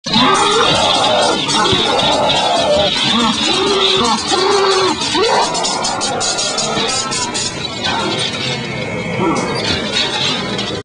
TheHorrorDome.com Psycho Haunted House Halloween Animatronic Prop
Kneeling, chained character savagely thrashes back and forth and up and down as he screams. Includes digital control, sound and amp with speaker. Requires air compressor. Ships freight. Additional $115.00 crating charge.